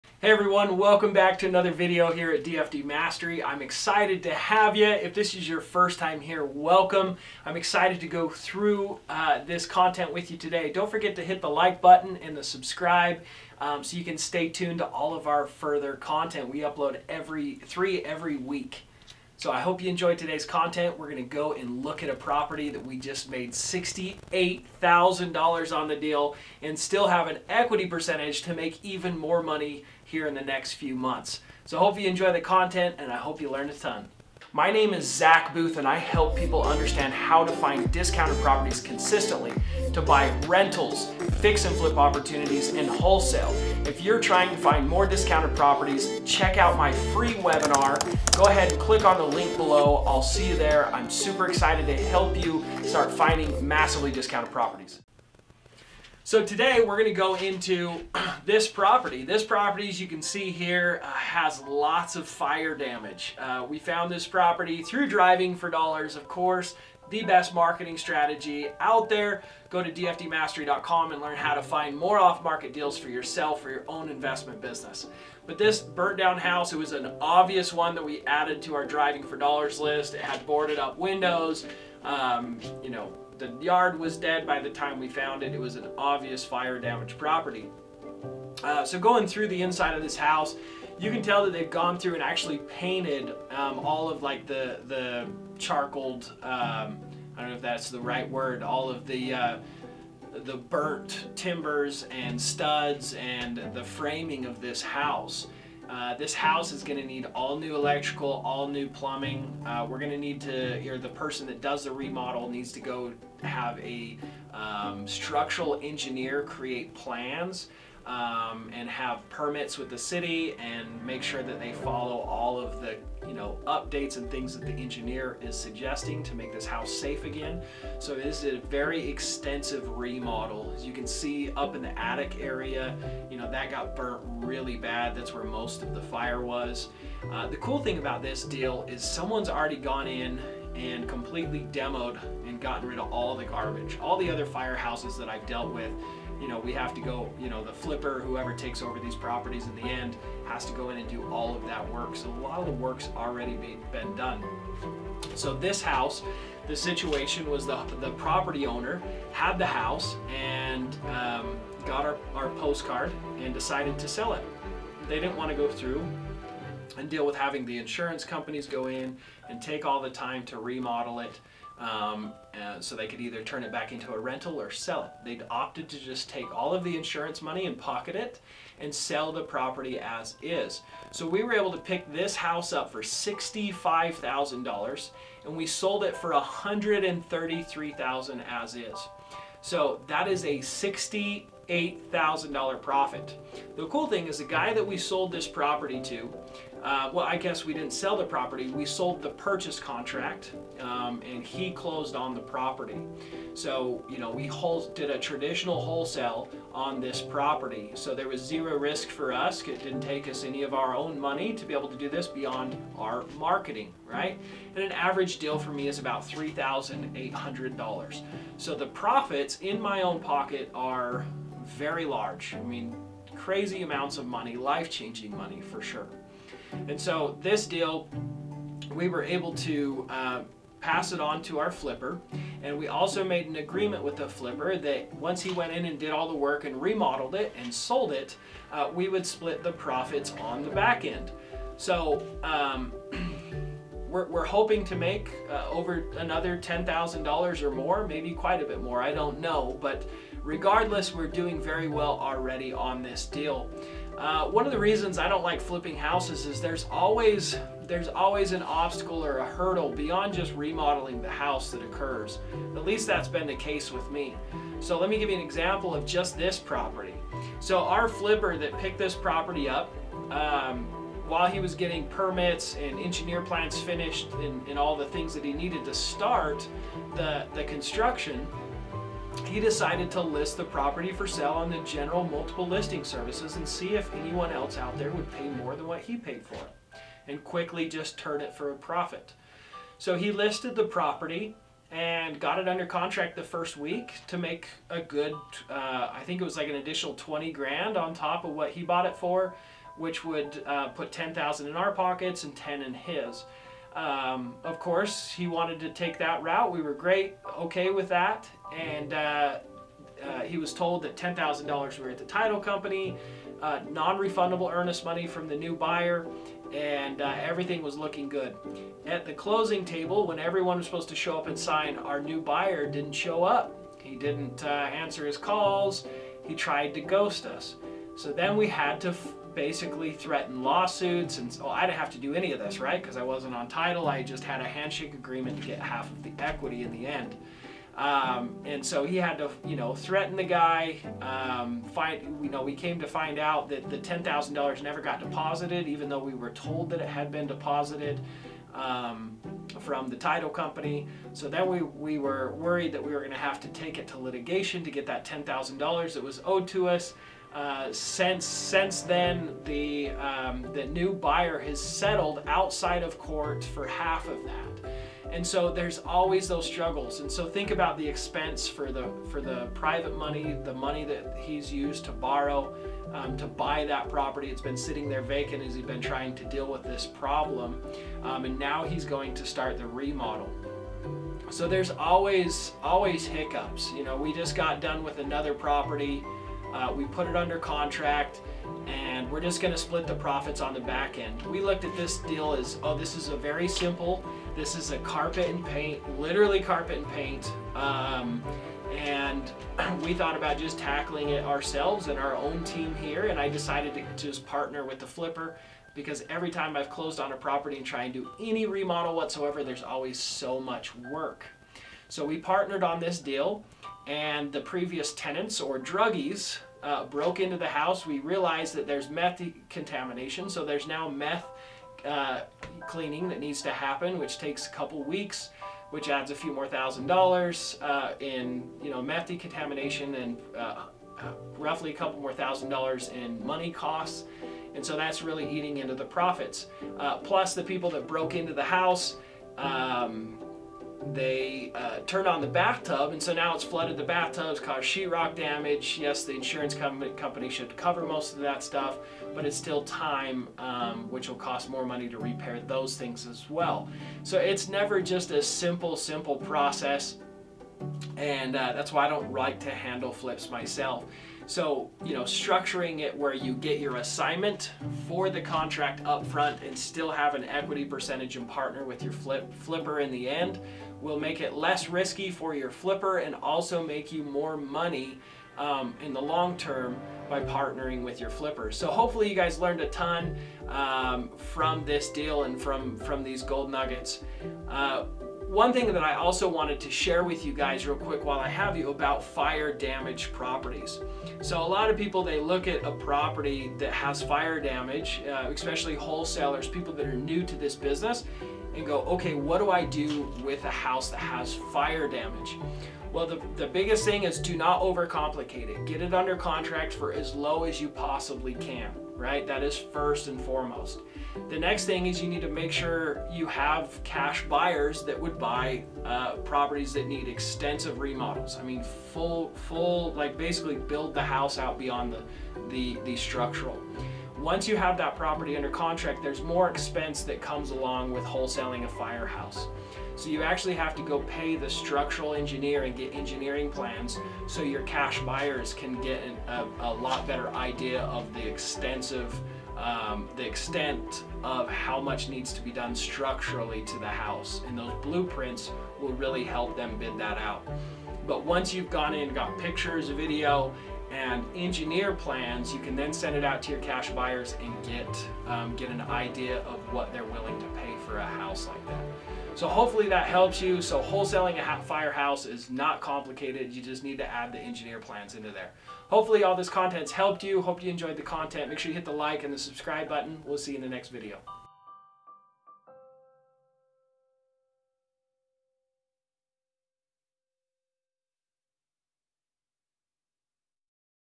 How to Make Money Wholesaling Fired Damaged Properties
I made $68,000 on a burned house, do you guys believe that? Watch this content! In this video, I talk about how to make a deal like this one and more specifically, how to make money on burned houses. Another important thing that I explain in this video is how to work with flippers and the importance to do it.